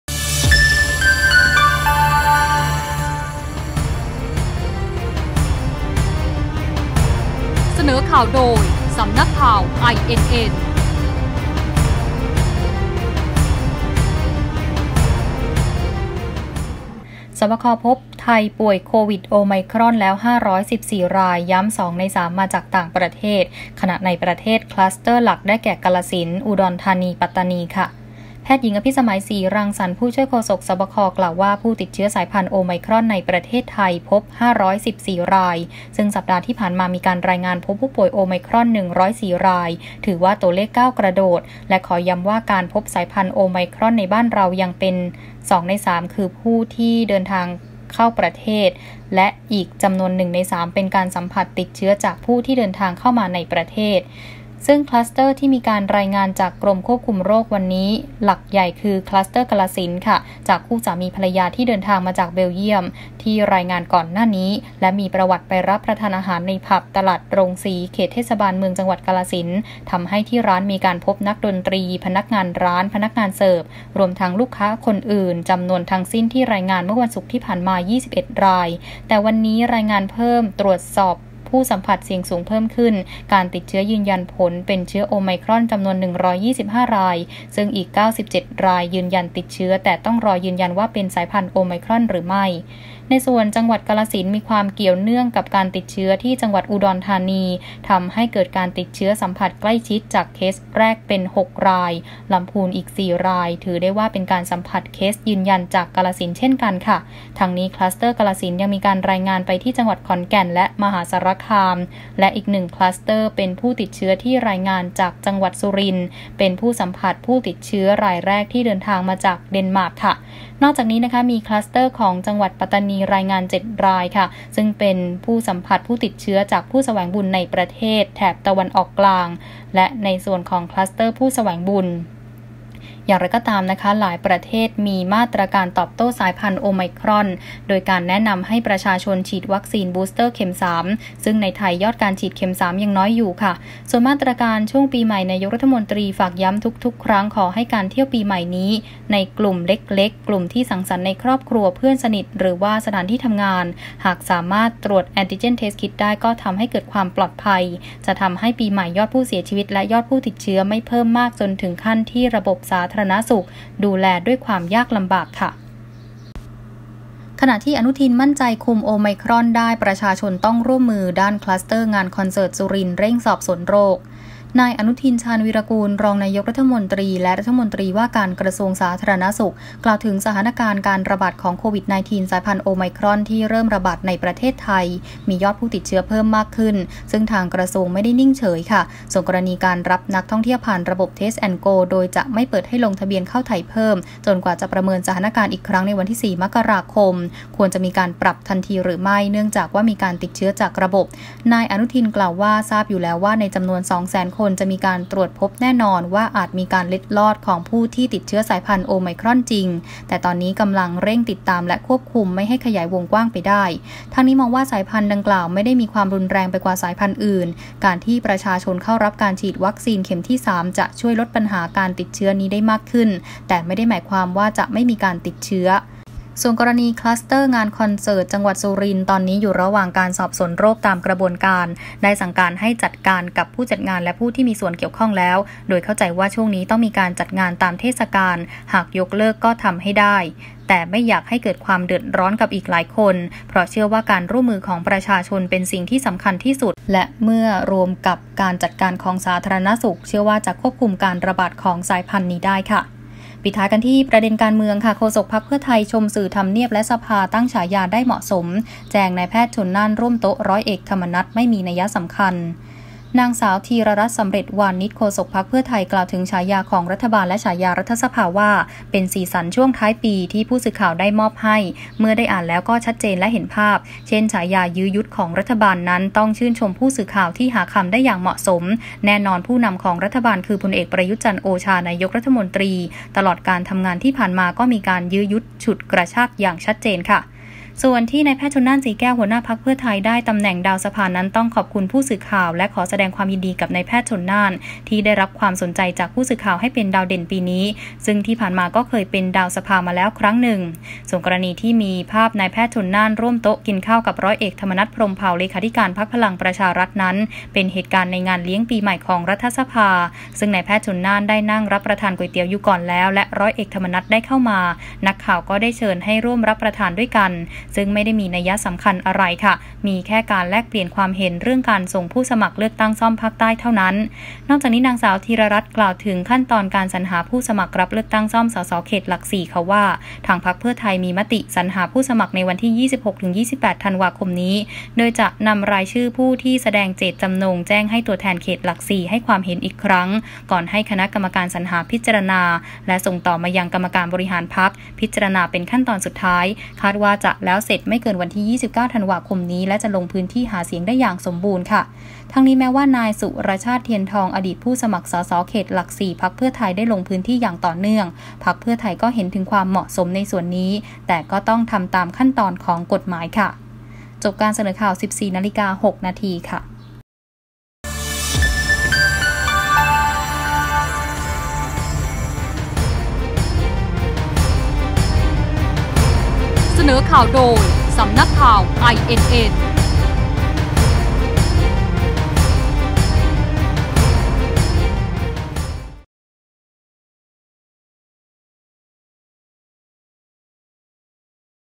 ศบค.พบโอไมครอนแล้ว514รายกาฬสินธุ์คลัสเตอร์หลัก : ข่าวต้นชั่วโมง 14.00 น.
-ศบค.พบโอไมครอนแล้ว514รายกาฬสินธุ์คลัสเตอร์หลัก
-พท.ชี้"ชลน่าน"ร่วมโต๊ะ"ธรรมนัส"ไม่มีนัยสำคัญ
- "อนุทิน"มั่นใจคุมโอไมครอนได้